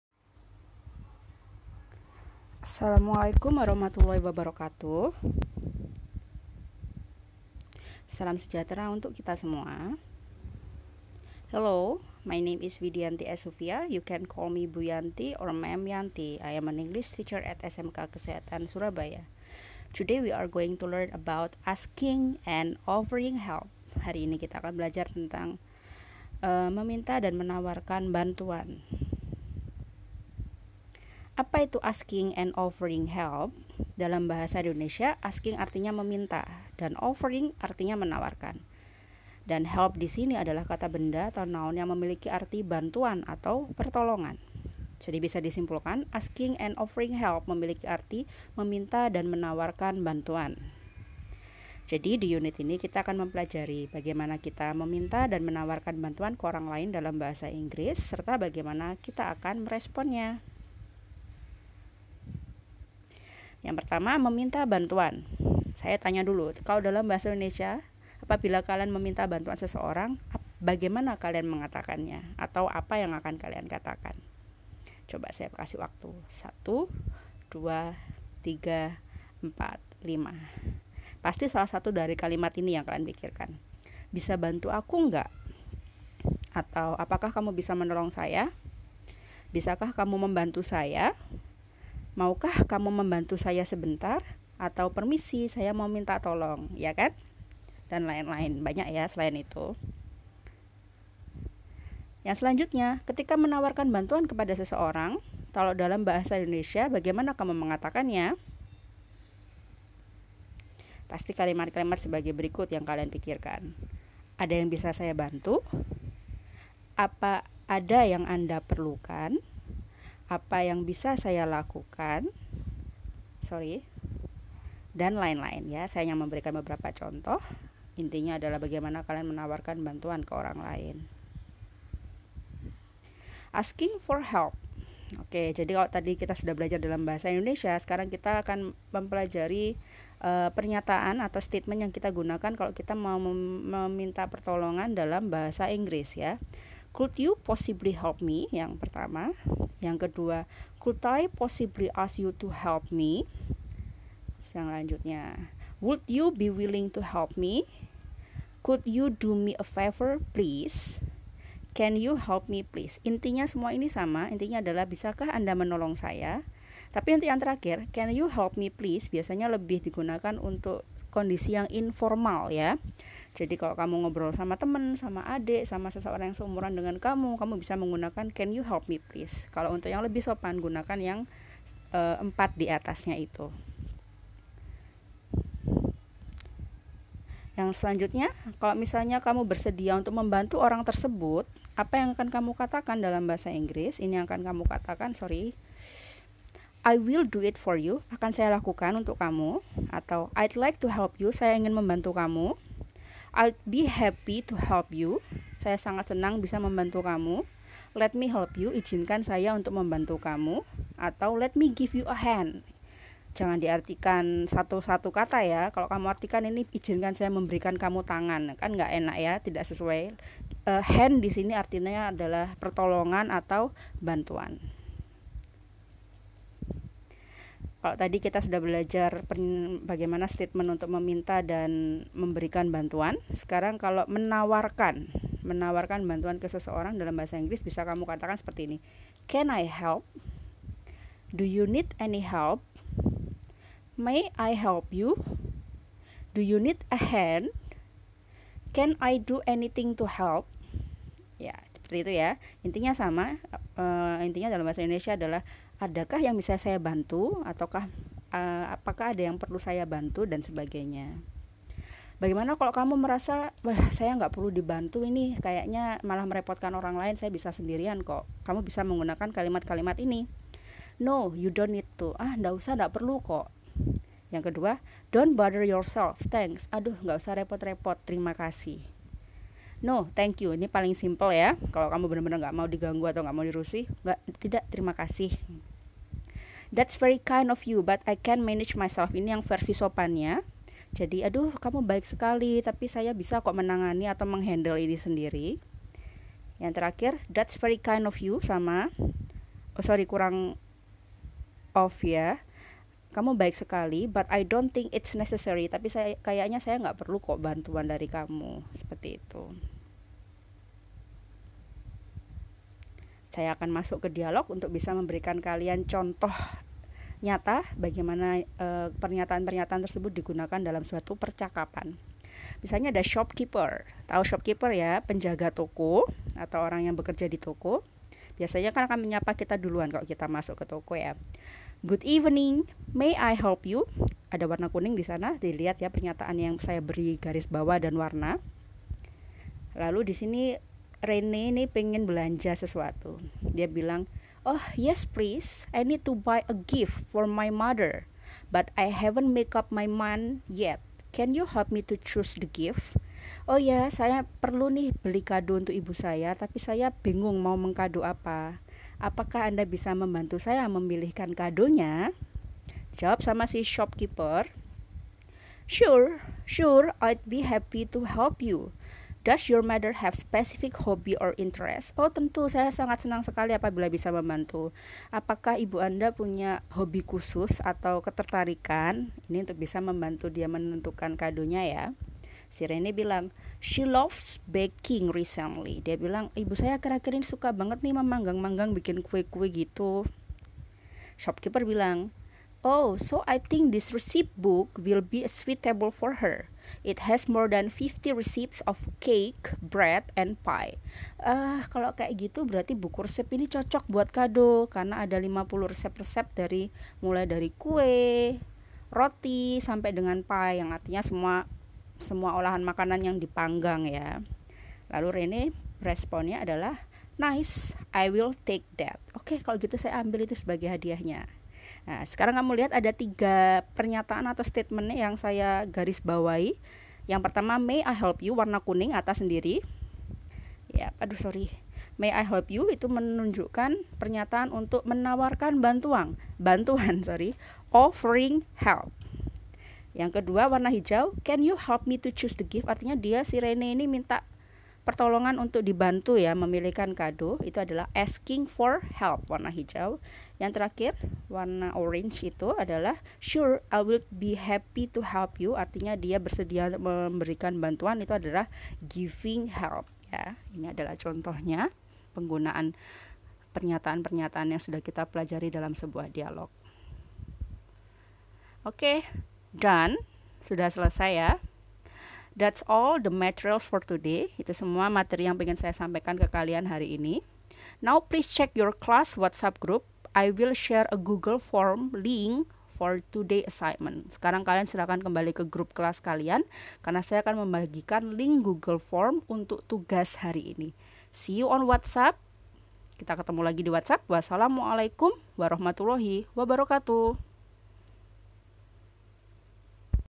Asking and Offering help - Meminta dan menawarkan Bantuan dalam Bahasa Inggris
Penjelasan materi Kelas XII SMK Unit 1 Bab Asking adn Offering help.